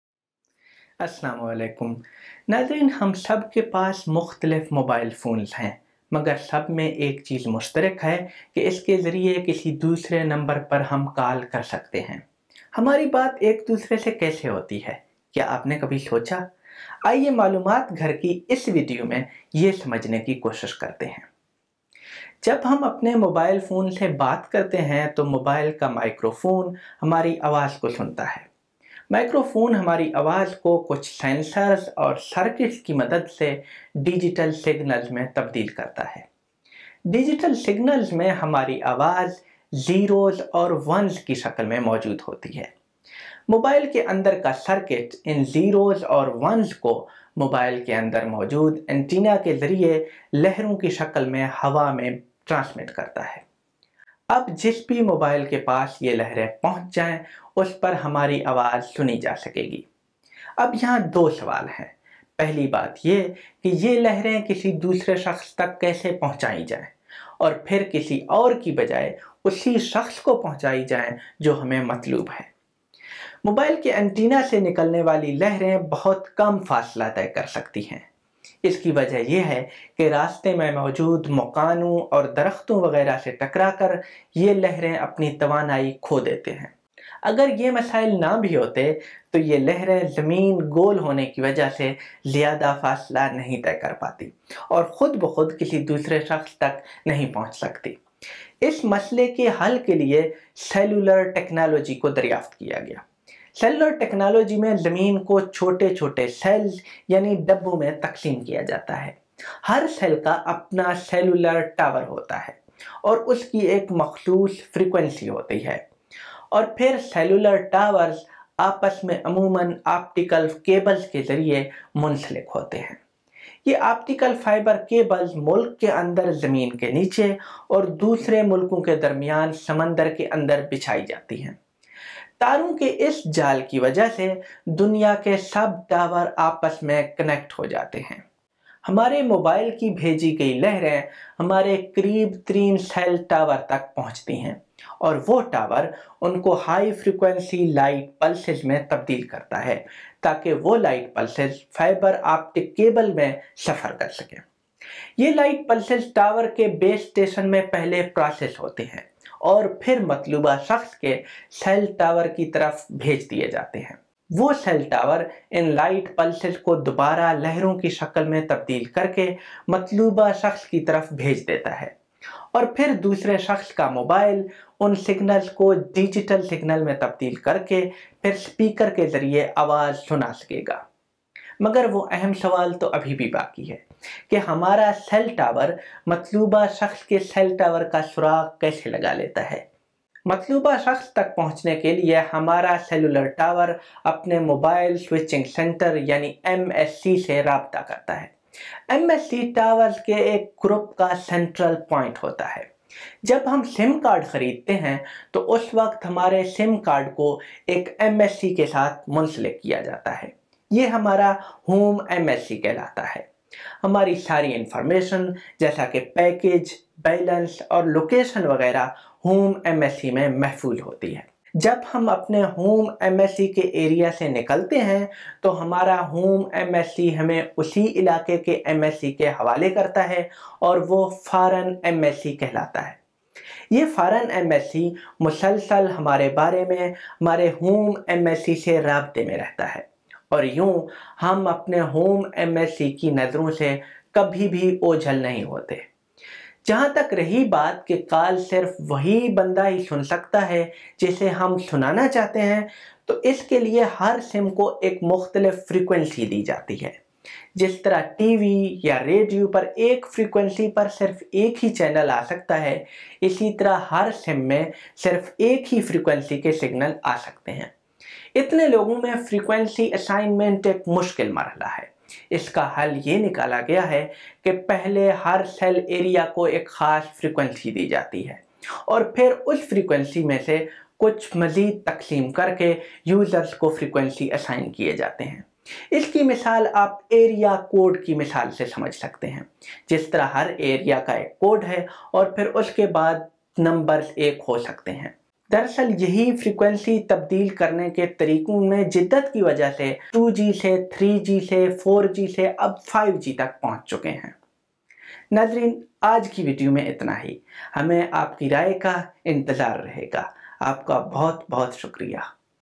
Working of Mobile Calling System Explained in Urdu
اس ویڈیو میں جانیں:
۔ موبائل سے کال کرنا کیسے ممکن ہے؟
۔ موبائل پر کی جانے والی کال کیا کوئی اور بھی سن سکتا ہے؟
۔ سیلولر نیٹورک کیسے کام کرتا ہے؟
۔ تھری جی فور جی اور فائیو جی میں فرق کیا ہے؟
۔ سم کا ڈیٹا کہاں سیو ہوتا ہے؟
۔ انٹرنیشنل کال کیسے ممکن ہے؟

In this video you will know:
- What is the working principle of cellular networks?
- How our call gets connected to the desired person?
- How cellular towers are connected?
- How international calls are possible?
- What is the mobile switching center (MSC)?
- Where record of our sim card is kept?
- How and why 3G, 4G, and 5G are different?

Special thanks:
Lateef Yaqoob
We all have different mobile phones but one thing we all have in common is that we can call another number through it.
Have you ever wondered how we talk to each other?
Let's try to understand this information in this home video.
When we talk on our cell phones, the cell phone microphone hears our voices.
The microphone converts our sound into digital signals with the help of some sensors and circuits.
In digital signals our sound is present in the form of zeros and onions.
The circuit inside the mobile transmits these zeros and wins in the form of waves through the antenna inside the mobile.
Now any mobile phone that receives these waves will be able to hear our voice.
Now there are two questions. The first is how to get these waves to another person and then to the person we want instead of someone else.
 Waves from mobile antennas can travel very short distances.
This is because the tidal waves that hit houses and trees along the way lose their energy.
Even if these problems did not exist, these waves would not be able to travel long distances because they are round in the ground and would not automatically reach another person.
 Cellular technology was discovered to solve this problem.
In cellular technology, the earth is divided into small cells (boxes).
Each cell has its own cellular tower and has a specific frequency.
And then the cellular towers are usually connected to each other by optical cables.
These optical fibers are traced underground within the country and in the sea between other countries.
Due to this network of wires, all the towers of the world are connected to each other.
The waves sent by our mobiles reach our nearest cell tower and that tower converts them into high frequency light pulses so that they can travel in optical fiber.
These light pulses are processed at the base station of the tower and then sent to the cell tower of the desired person.
The cell tower converts the light pulses back into waves and sends them to the desired person.
And then the other person's mobile converts these signals into digital signals and then the sound can be heard through the speaker.

But the important question remains, how does our cell tower track the cell tower of the desired person?
To reach the desired person, the cell tower takes help from the Mobile Switching Center (MSC) in their area.
MSE is the central point of a group of cell towers.
When we buy a SIM card, our SIM card is assigned an MSc.
This is called our home MSE.
All our information such as package, balance and location is stored in Home MSE.
When we leave the area of ​​our Home MSE, our Home MS assigns us to the MSc of that area, it is called Foreign MSc.
 This Foreign MSc is in constant touch with our Home MSc about us and so we never hide from the eyes of our Home MSc.
As far as the call can be heard only by the servant we want to hear, each SIM is given a different frequency.
Just as there can be only one channel on a frequency on a TV or radio, there can be only one frequency signal on each SIM.
Frequency assignment is a difficult step for so many people.
The solution is to first assign a specific frequency to each cell area and then divide that frequency into quizzers.
In fact, it is by changing the methods of frequency distribution that we are moving from 2G to 3G, 4G and 5G.